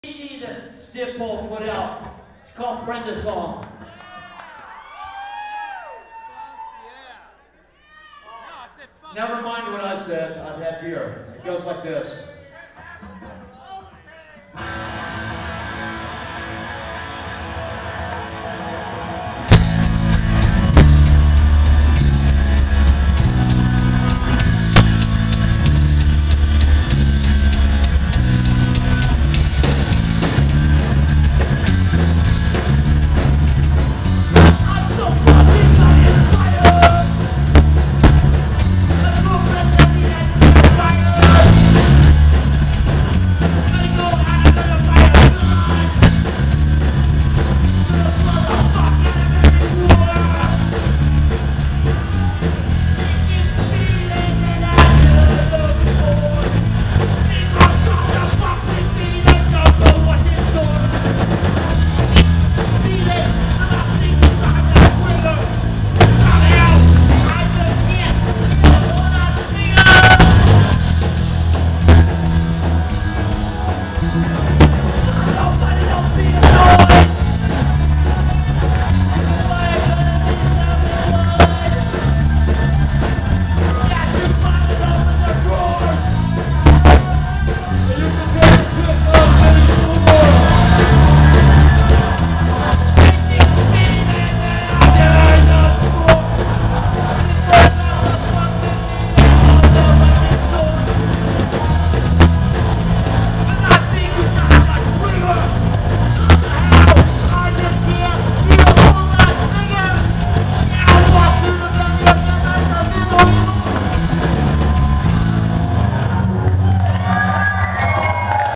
Pink Lincolns 11-6-10 at the State for Stiff Pole Memorial Show pt 2
Another Pink Lincolns song I shot at the Stiff Pole Memorial Show at the State on 11-6-10. Video by Eric Grant